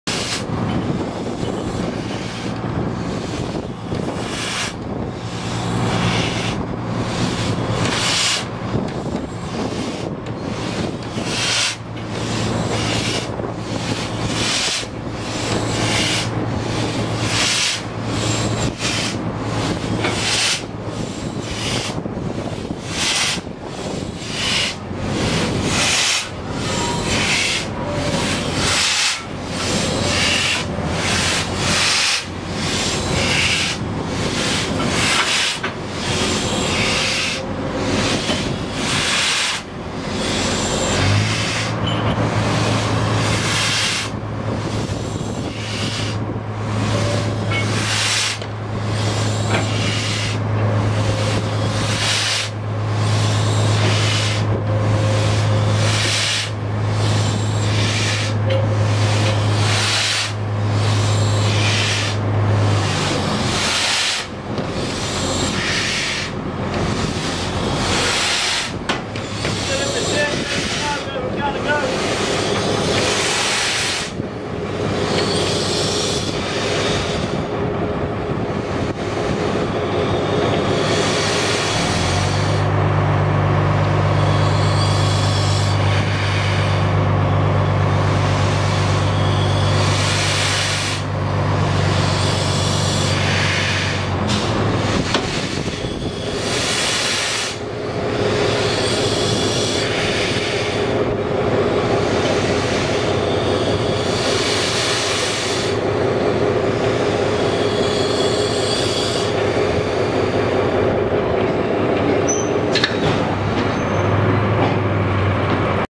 Shunting NZR Jb1236 at Mainline Steam Plimmerton's Yard
Shunting Ian Welsh's NZR Jb1236 at Mainline Steam Plimmerton's Yard